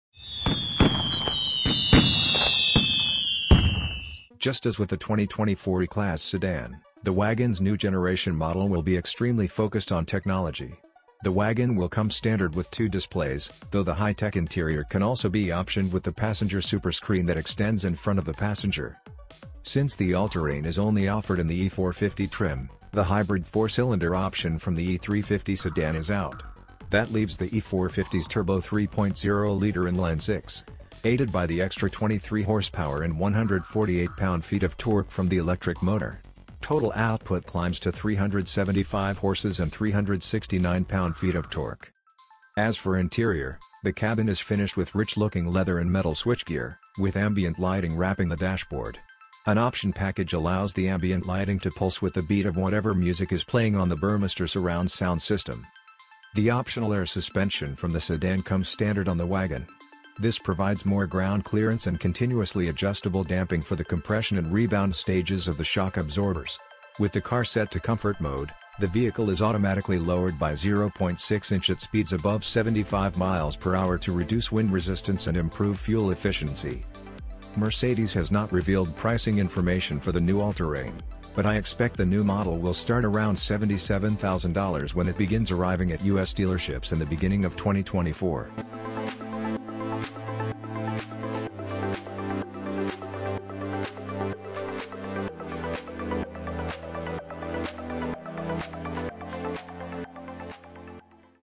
2024 Mercedes-Benz E-Class All-Terrain - Fast Review!
2024 Mercedes-Benz E-Class All-Terrain - Fast Review!

The All-Terrain comes as the hybrid E450 4Matic with a turbocharged 3.0-liter inline-six engine making 375 hp from 5,800 to 6,100 rpm and 369 torque. The electric Boost system provides 23 hp and 151 lb-ft.

Approximate price is about 77.000$.
In the U.S. Dealers in the beginning of 2023.